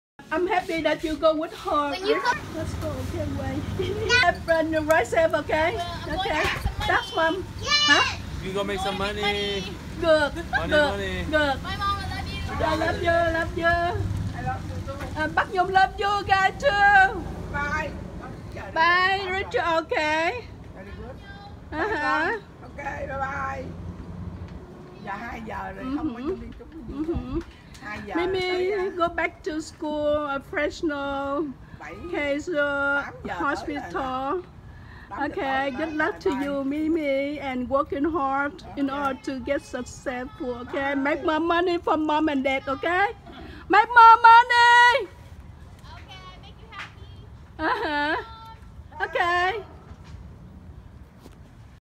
6/3018 Katherine/ Mimi went to Fresno for another year PGY1!
Dear Mimi,  as you went to Fresno where you need to work for your PGY1 Residency with Kaiser Permanente/ Fresno Medical Center.

We already miss you very much.  Take good care of yourself.  Love you always!

Video by Katherine's mom, Tuyet Tran.